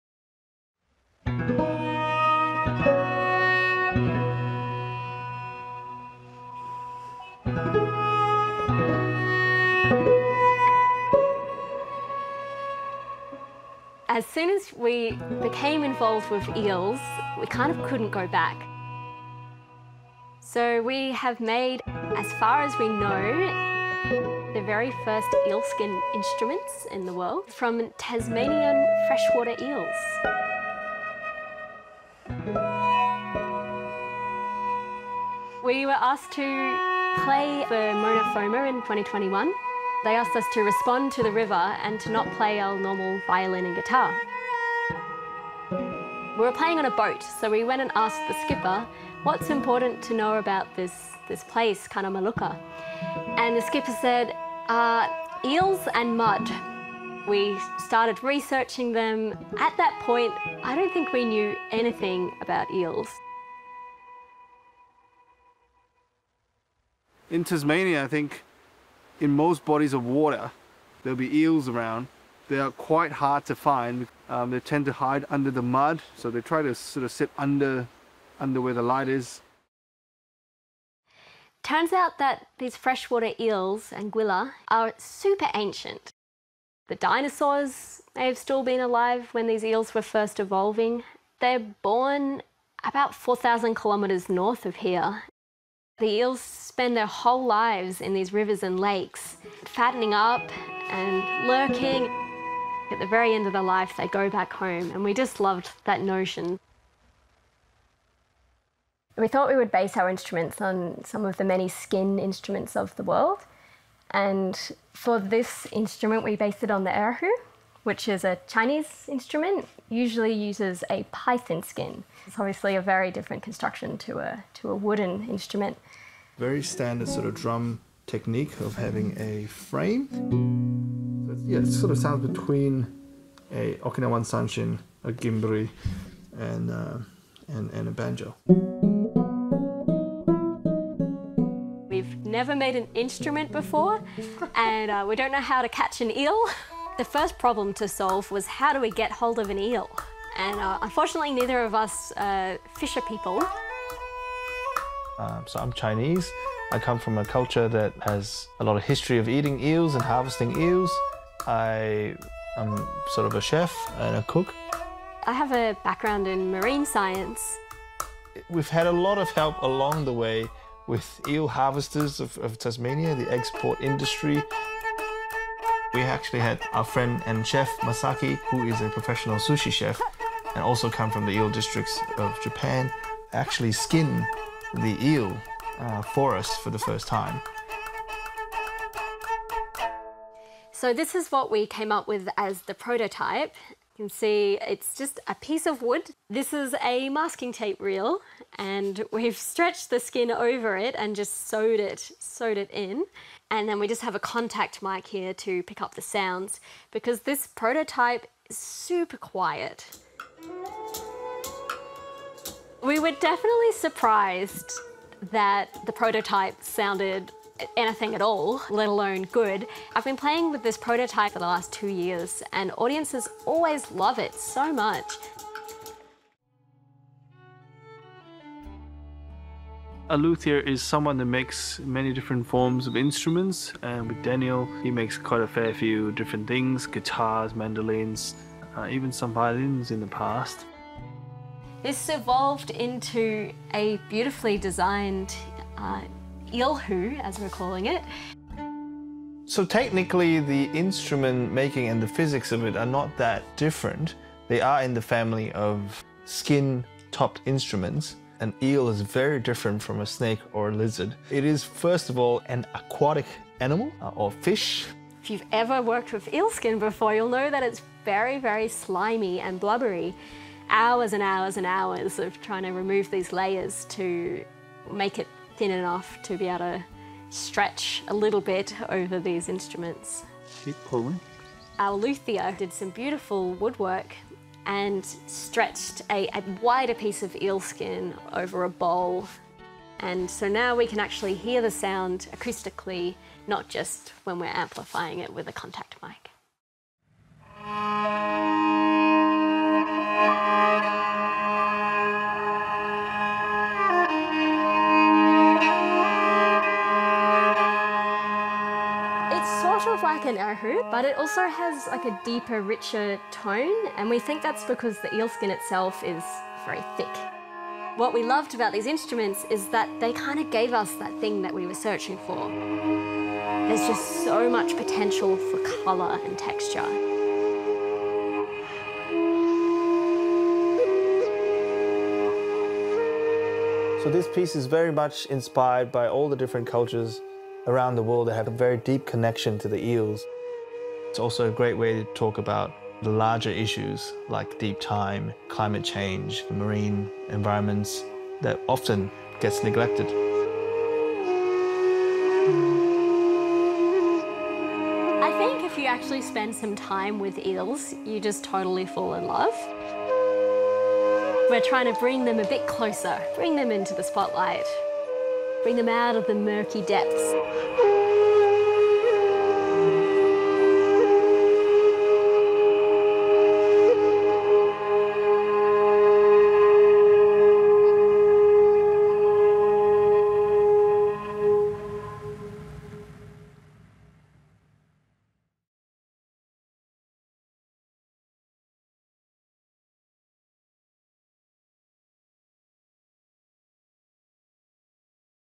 Are these the first eel-skin instruments in the world? | Art Works
Yyan Ng and Emily Sheppard are Tasmanian artists and musicians who have created what could be the first eel-skin instruments in the world.